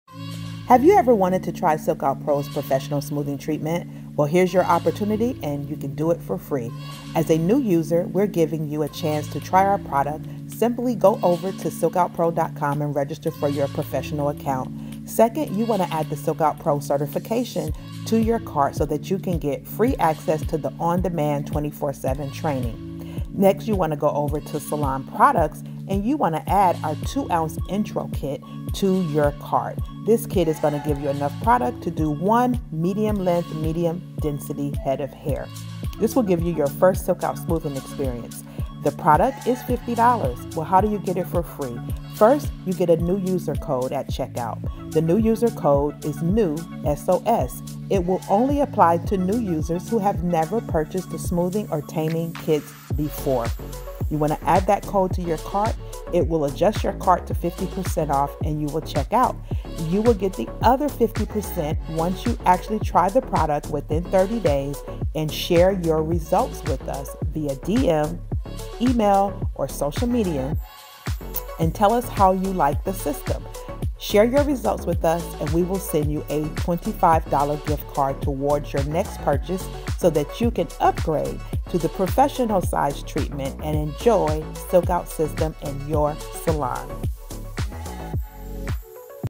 SilkOut Smoothing Treatment Sampler Kit!
For a limited time, we are giving new professional users a chance to try our intro kit for FREE.
1.
2. Add our on demand FREE certification class.
3. Purchase our 2oz Sampler Kit with code NEWSOS.
4. Use and share your results via email, social media,  or dm us within 30 days to get $25 gift card towards your next purchase.

Offer valid for next 50 new users.